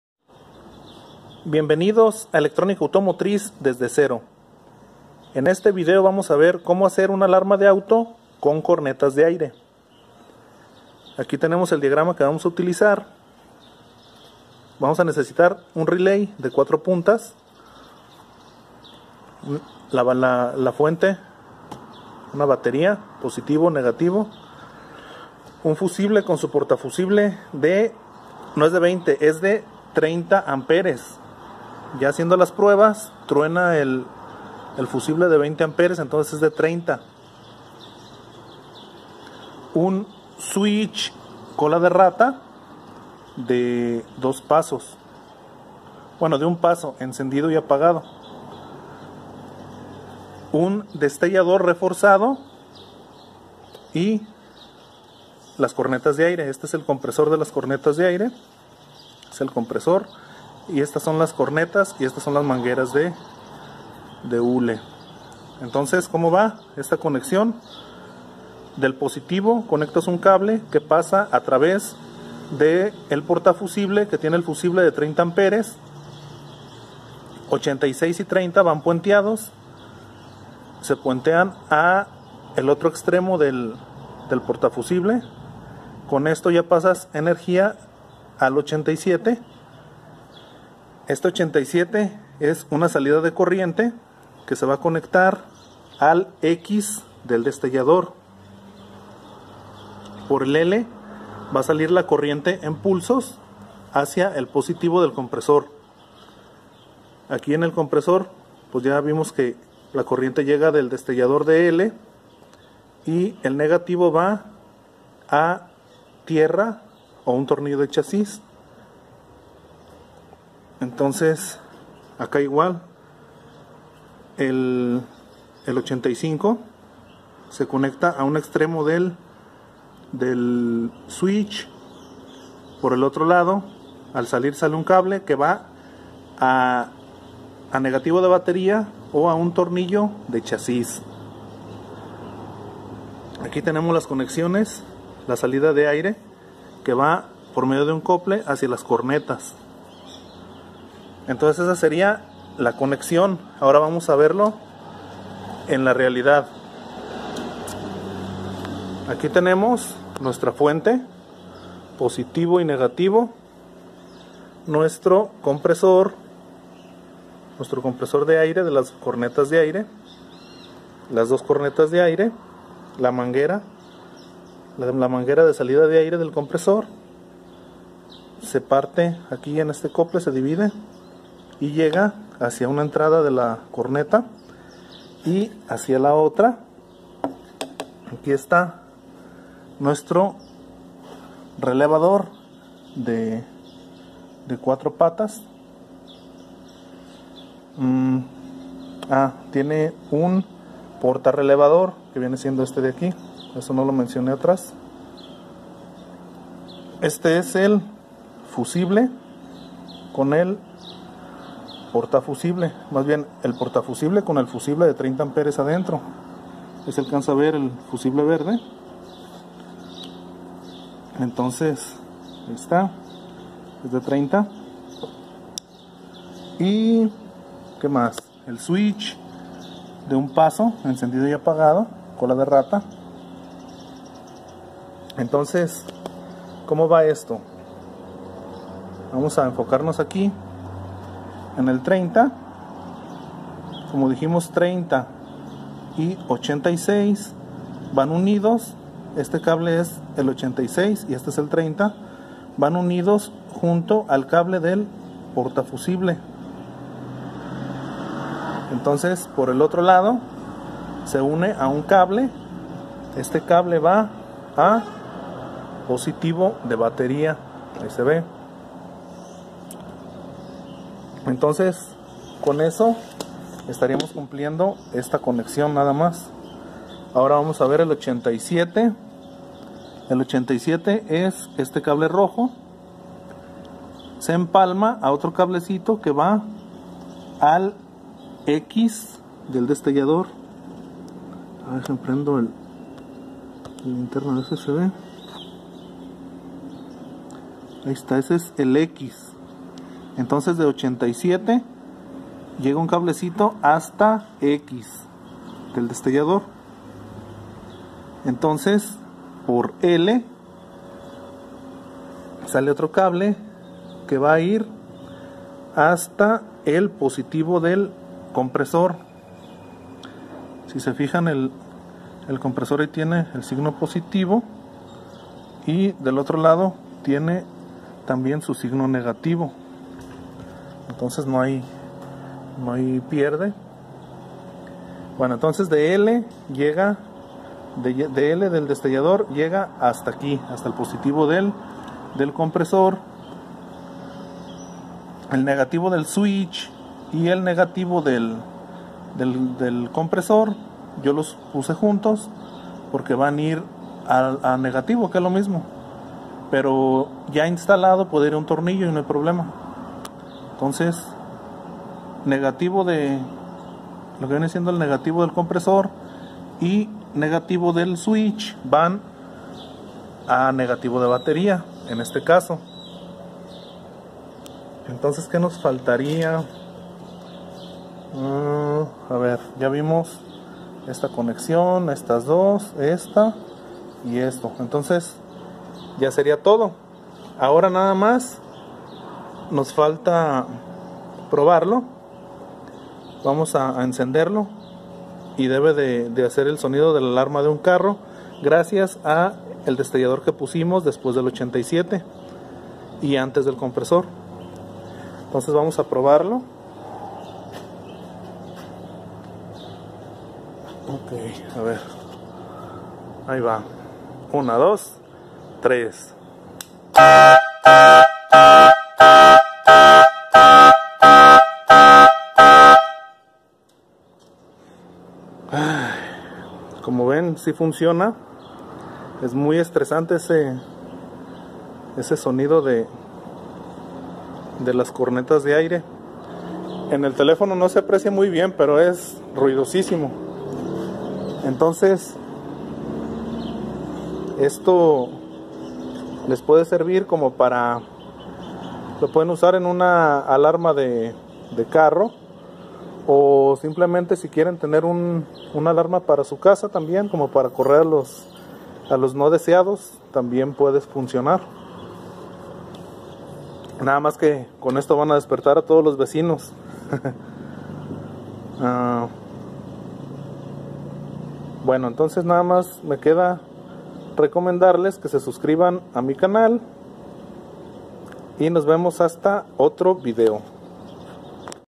COMO HACER ALARMA DE CARRO
en este video te enseño a hacer una alarma de carro